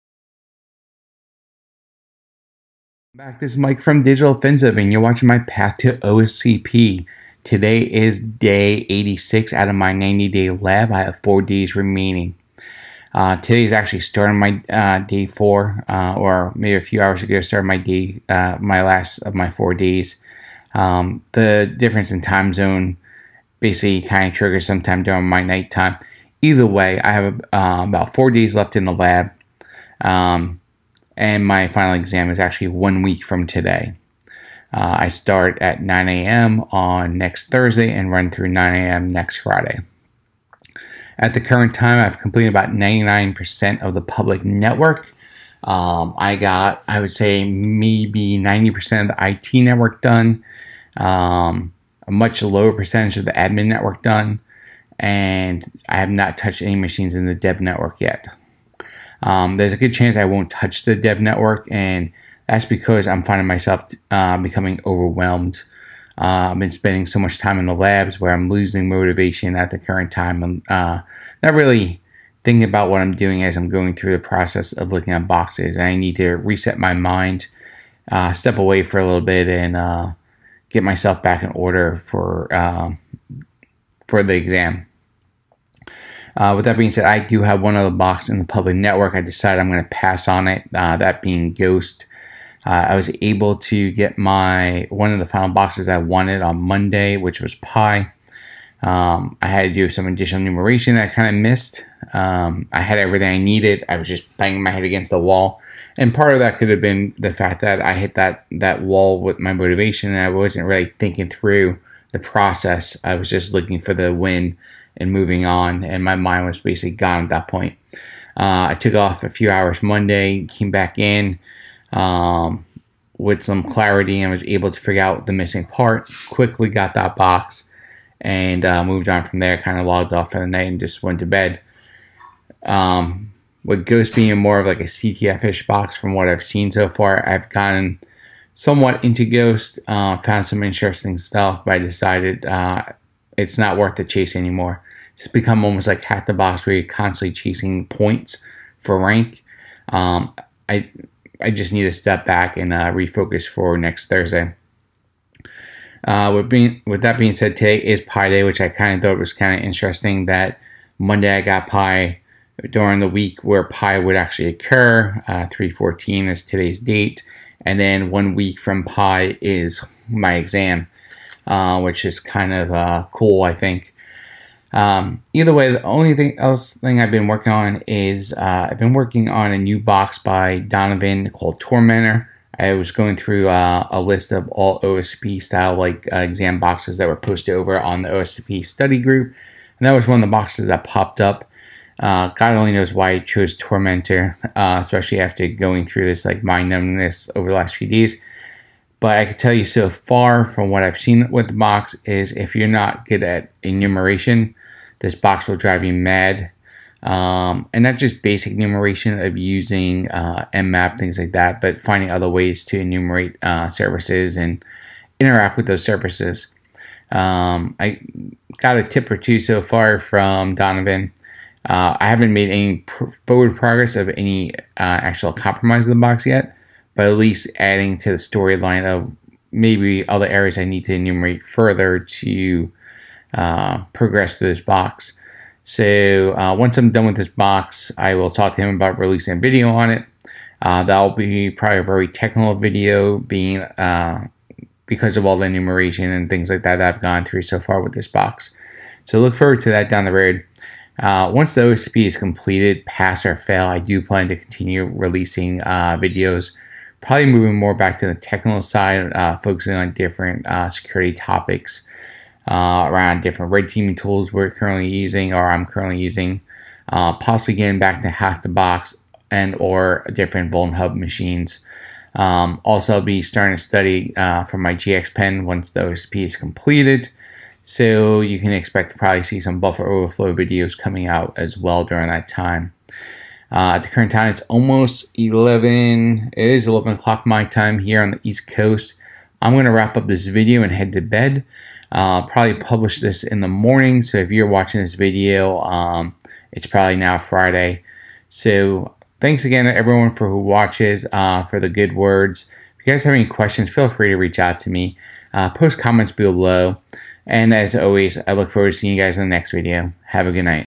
Path to OSCP days 80 -86
Part of my Path to OSCP series. I will be documenting my lab time to help others progress through the labs. Giving tips and encouragement along the way. Today I completed days  80 - 86. As of now I have completed 99% of the public network. I have unlocked all the networks. I have completed 90% of the IT,  about 30% of the admin network and 0% of the Dev network. On this past Monday, I completed my last goal other than passing the exam. I finished the public machines that I wanted with the completion of PIE. Which is interesting as PIE day was Thursday which is a week before my exam. In the video, I cover lack of motivation, my final prep, next steps and what's to come.